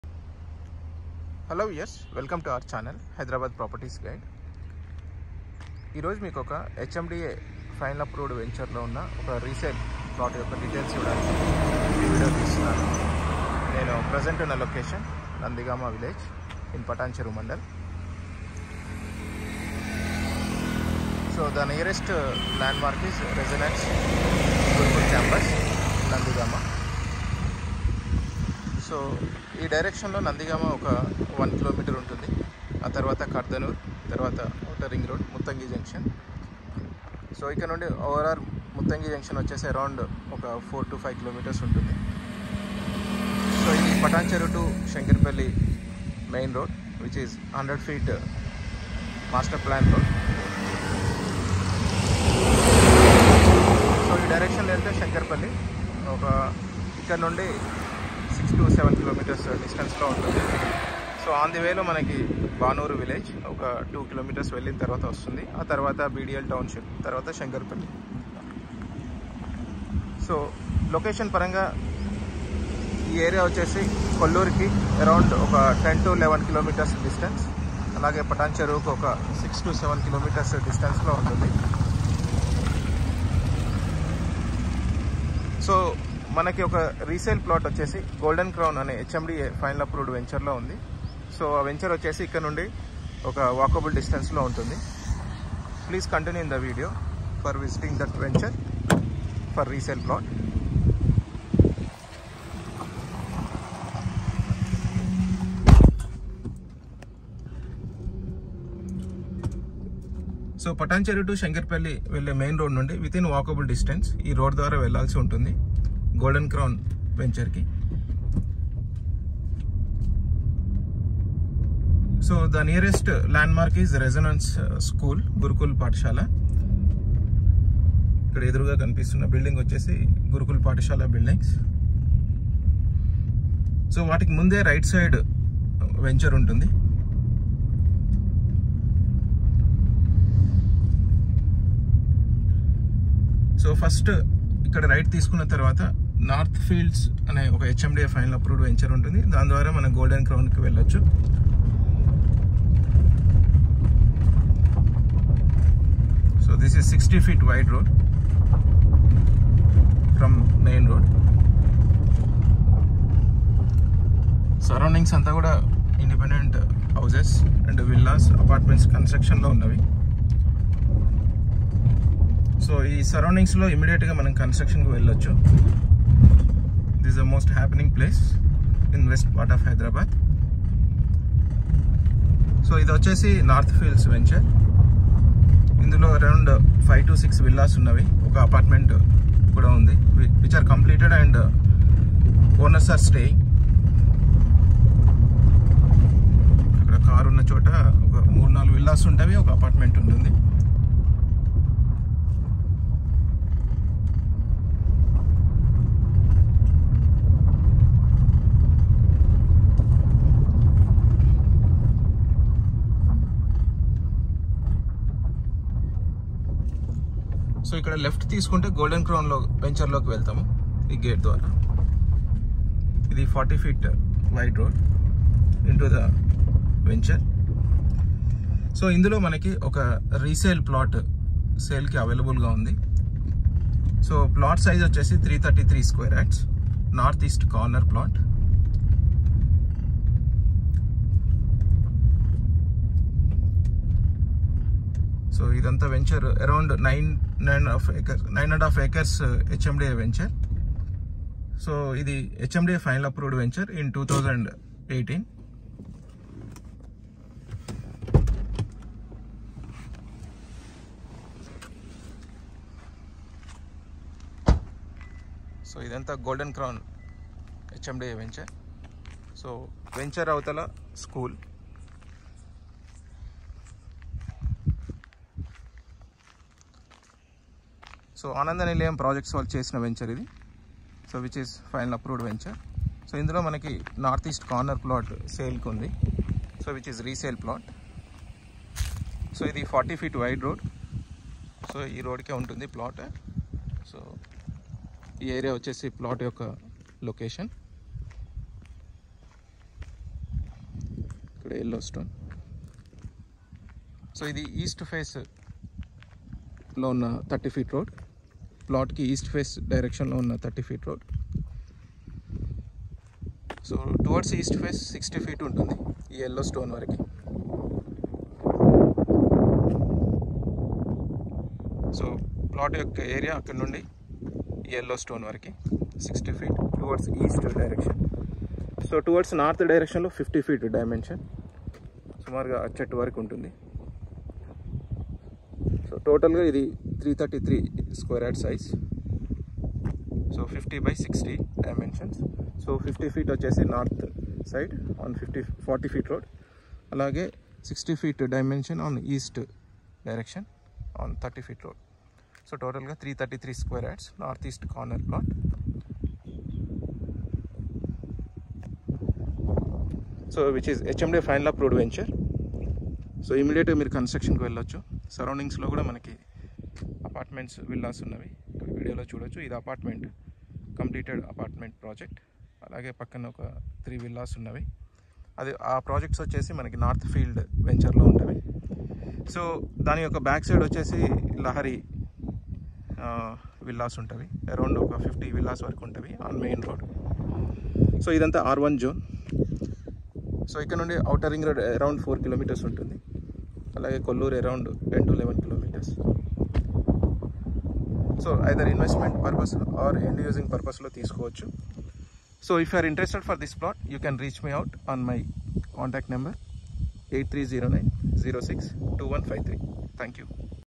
NORTH-EAST CORNER PLOT FOR SALE IN A HMDA FINAL APPROVED LAYOUT @ NANDIGAMA | PATANCHERU | HYDERABAD
** PLOT & LOCATION DETAILS:
1. Final Approved HMDA layout at Nandigama in Patancheru Mandal
2. 2 KMs distance from Vishaka X Road and 4 KMs away from ORR Exit #3
3. Project Name: GOLDEN CROWN by ANANDANILAYAM Developers
4. Plot size: 333 sq.yd(50*60) with North-East facing
5. Well developed HMDA project with Overhead Electricity, Underground Drainage, Water Overhead tank and Openspace for Parks and Social-Infra.
6. Ready to construction for House
7. Clear titled Project
8. 15min drive from ORR Exit #2, Kollur
9. 5 KMs from Railway Goods Yard, and Medha Rail Coach Factory.
10. Near to OUTER RING ROAD.
11. 20 to 30 minutes Drive from FINANCIAL DISTRICT,KOKAPET,NANAKRAMGUDA,GACHIBOWLI & HYTEC CITY.
12. Its in R1 ZONE.

** PRICE:
   Rs. 34,000/- per sq.yd(Slightly Negotiable)